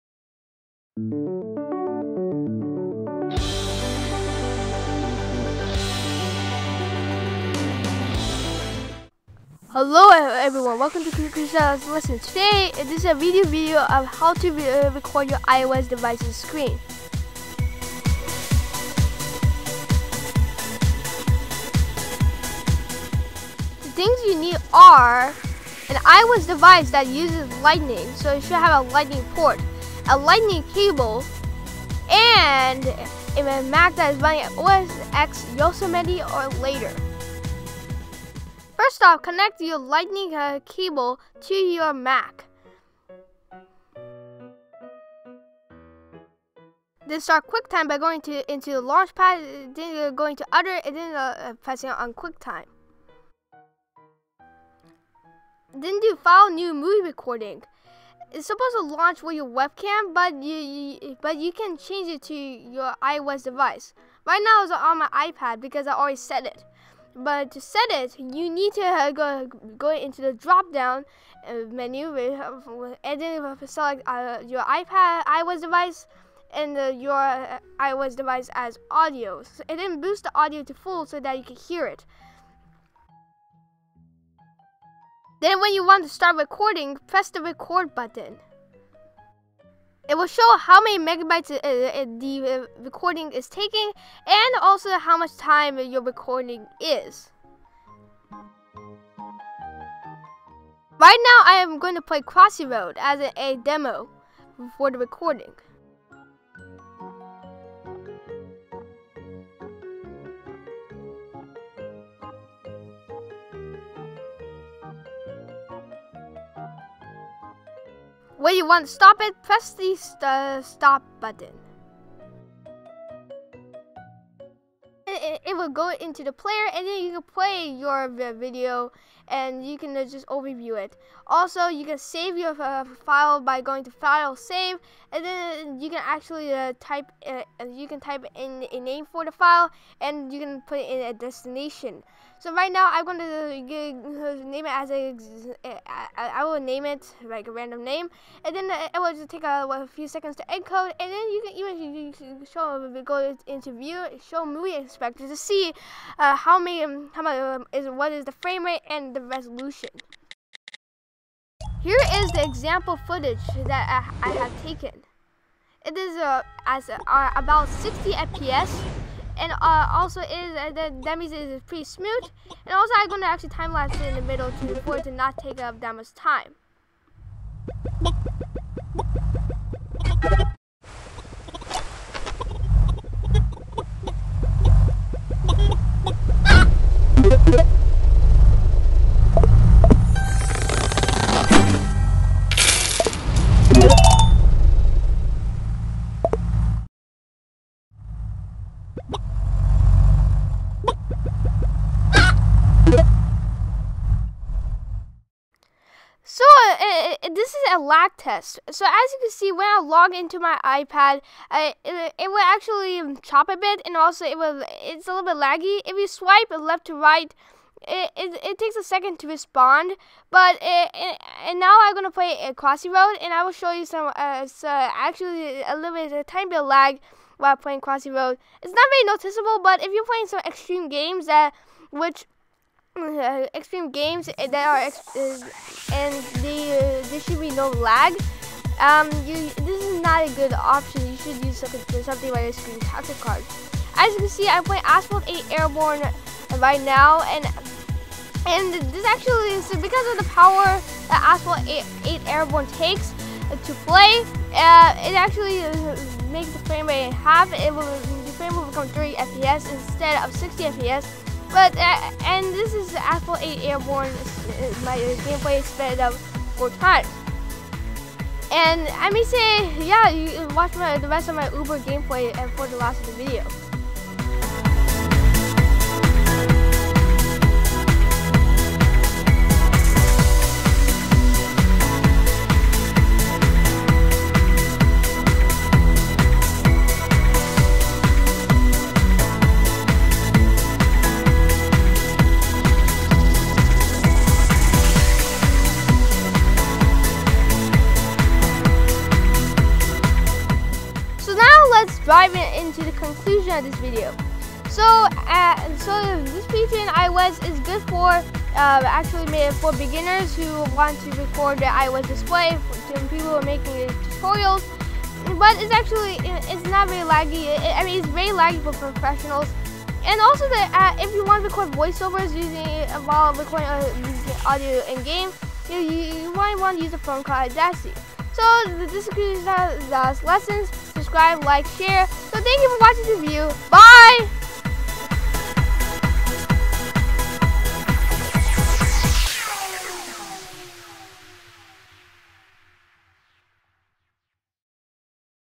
How to Record Your iOS Device's Screen
This video would show you how to record your iOS device's screen. This is only compatible with iOS devices that have a lightning port, and you need OS X Yosemite or later for this to work. We use Quicktime in this video. This was a redo of a video that I made about the exact same topic.